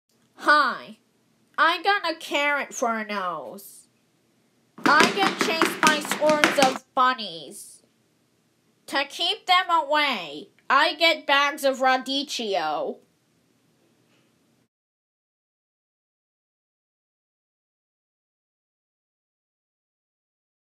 Carrot Nose Guy (short)
This guy unexplainably has a carrot for a nose. Which makes his life difficult.